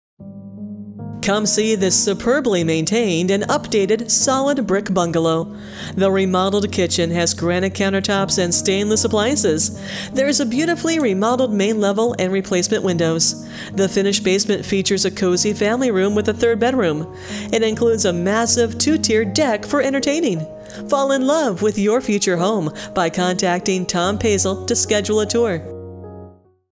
245 Spruce Lane Chicago Heights, IL | ColdwellBankerHomes.com
245 Spruce Lane, Chicago Heights, IL

A PERFECT “11”- SUPERBLY MAINTAINED AND UPDATED SOLID, BRICK BUNGALOW WITH ALL THE BELLS AND WHISTLES- OVERSIZED 2-1/2 CAR GARAGE- FURNACE 2020- AIR 2021- ROOF/LEAF GUARD APROX. 10 YEARS- REMODELD KITCHEN WITH GRANITE COUNTER TOPS, CERAMIC FLOORING/ BACKSPLASH, AND STAINLESS APPLIANCES- BEAUTIFULLY